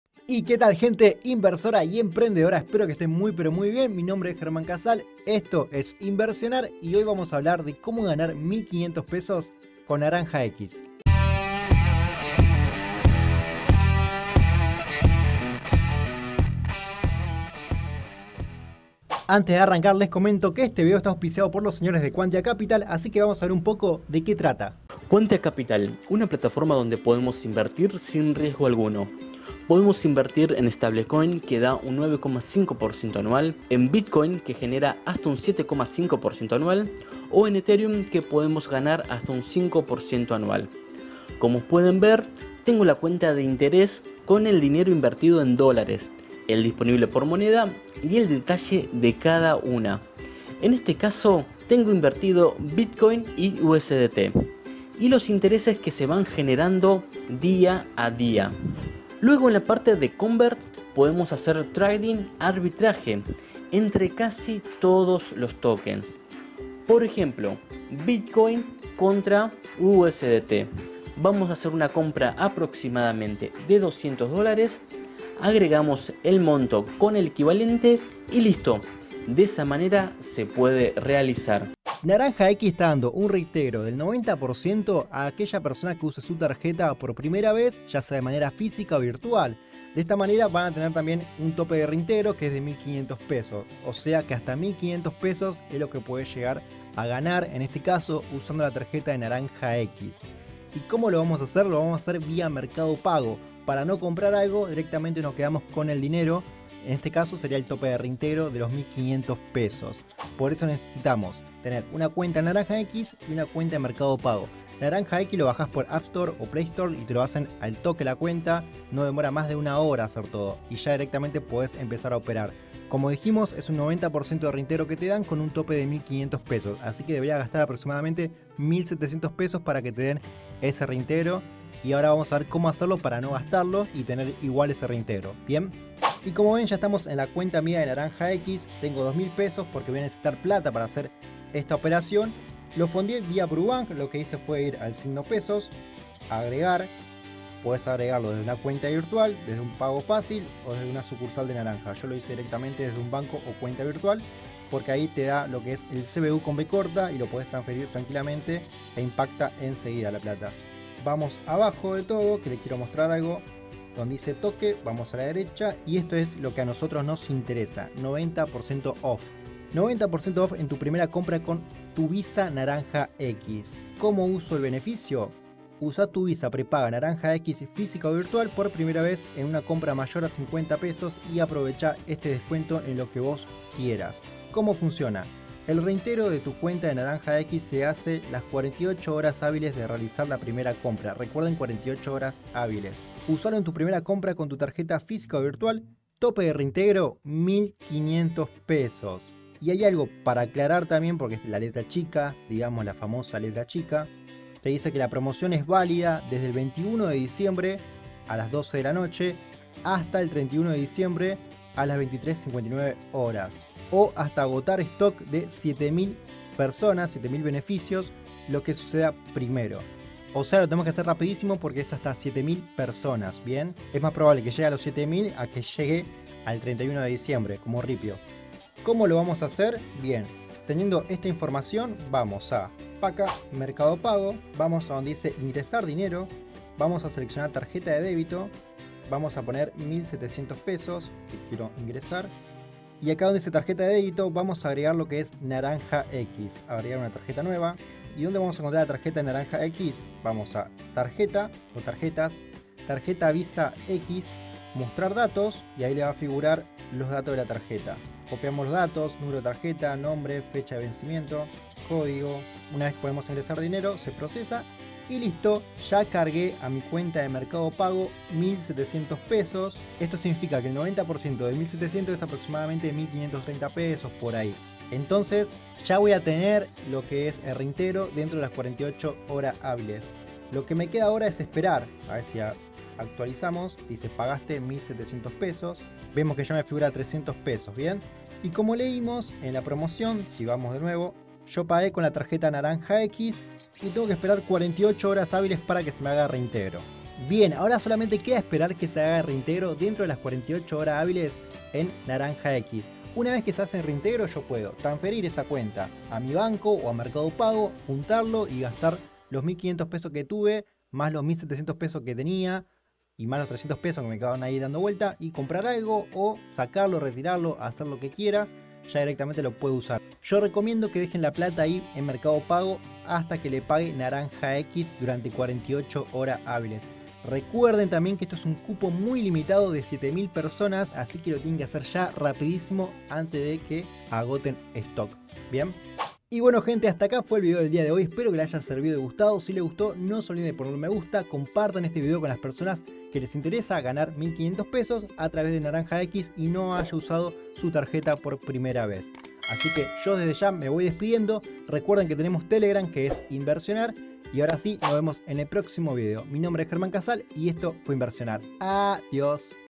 APURATE NARANJA X REGALA $1500 ❗ GANA DINERO DESDE CASA🤑
link para difusión

QUE TAL GENTE INVERSORA Y EMPRENDEDORA MI NOMBRE ES GERMAN CASAL ESTO ES INVERSIONAR Y HOY VAMOS A HABLAR DE COMO GANAR DINERO CON NARANJA X  Y MERCADOPAGO